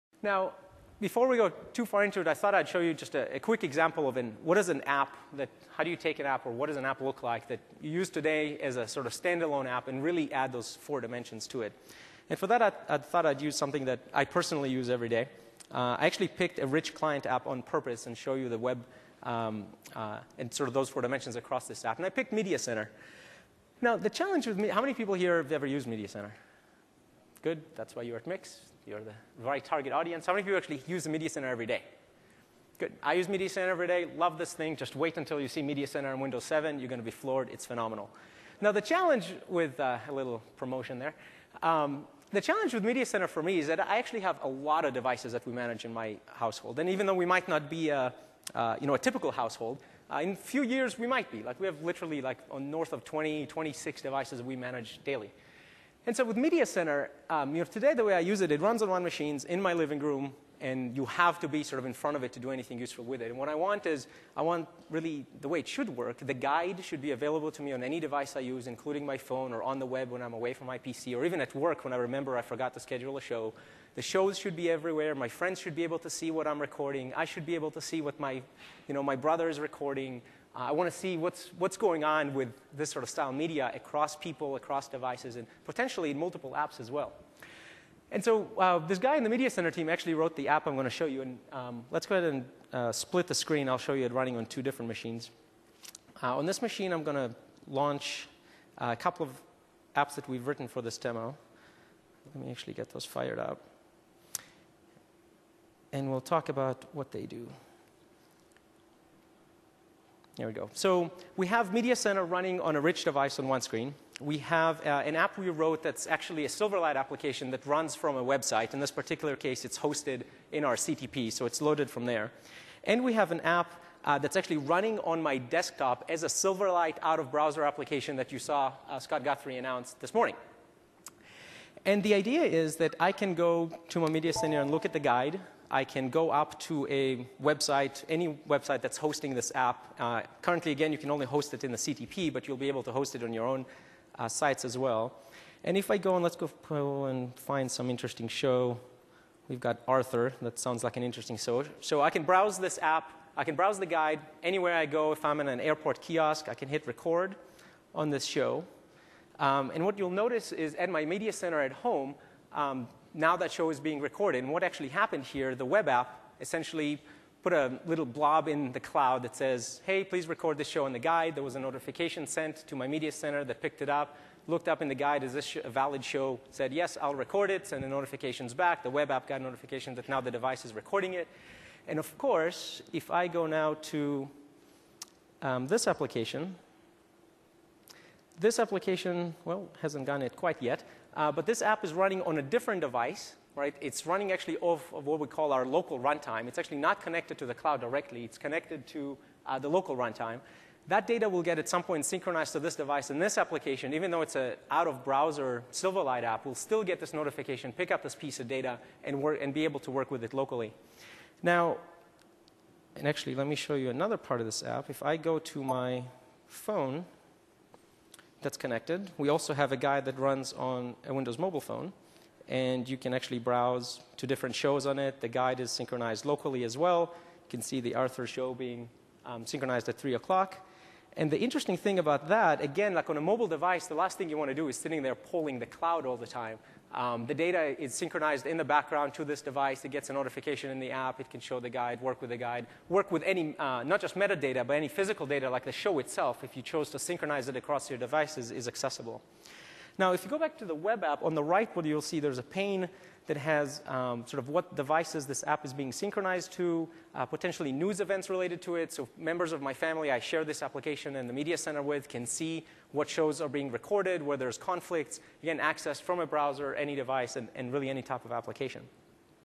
Aplicativo Live Mesh com o Windows Media Center
Esse vídeo mostra o potencial da plataforma Live Services através de um aplicatiovo Live Mesh. O guia de programação eletrônico do Windows Media Center é sincronizado através de celular, PC e Web e mostra como é possível agendar uma gravação remota num PC através do Live Mesh.